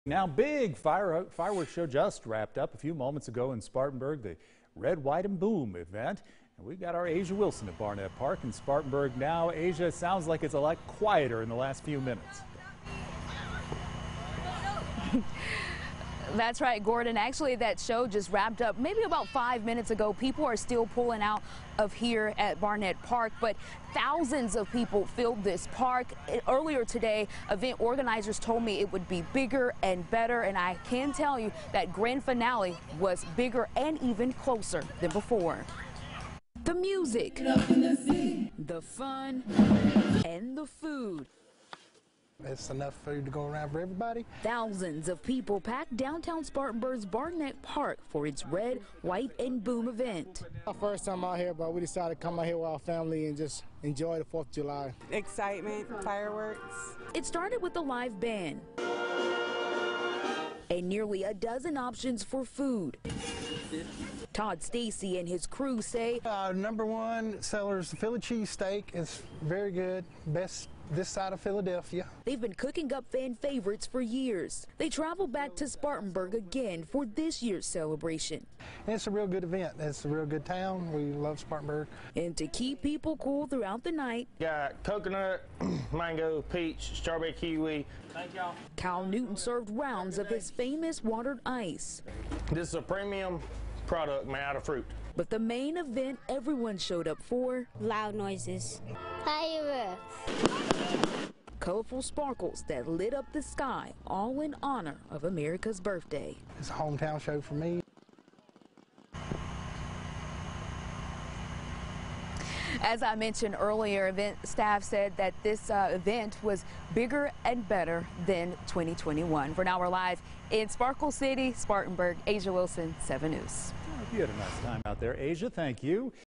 "Bigger and Better": Thousands show up for Spartanburg's Red, White & Boom event
Spartanburg held its Red, White, and Boom event on Monday night.